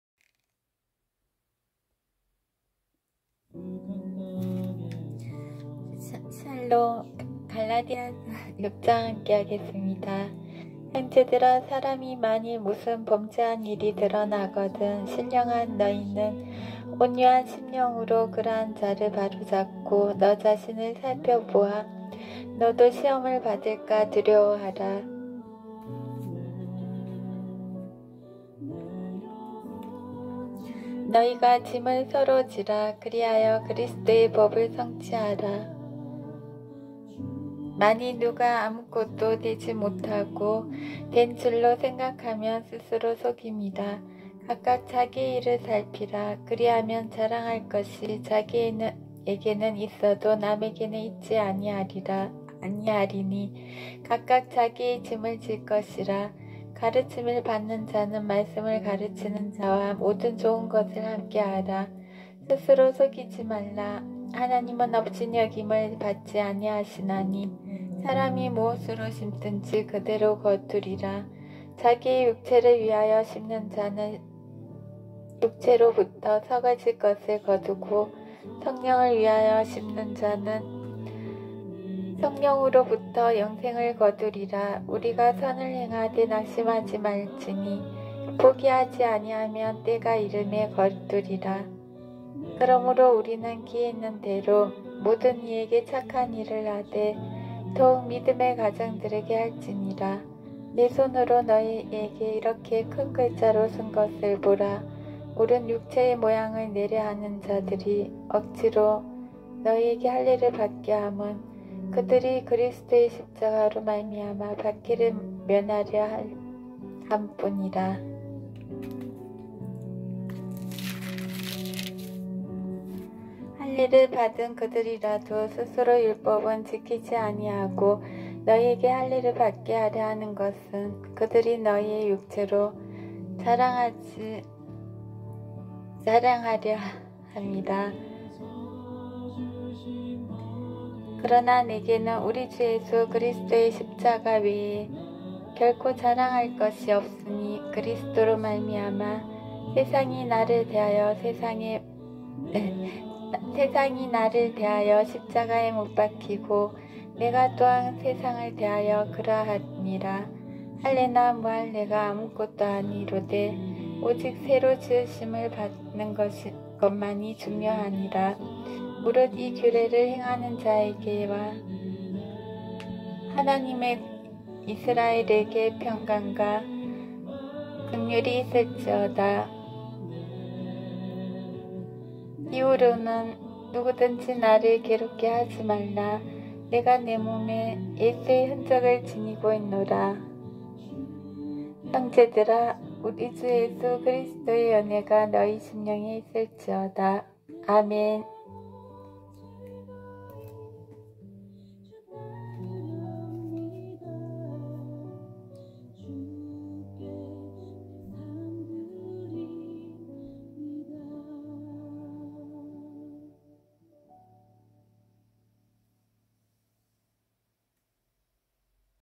🌷갈라디아서 6장 말씀 🌷
🌈2024년 1월 30일 화요일 오후🤗
2024년 1월 마지막주 저희 교회 암송구절 이에요🌸
스푼 김인수님 프사 말씀이 은혜되어 올렸어요🌸
은혜로운 말씀 들으시고✨️행복한 새 봄✨️맞이하세요 항상 영육간에 강건하시길 기도드립니다🙏모든분들 축복합니다 샬롬💖🙆‍♀️⚘️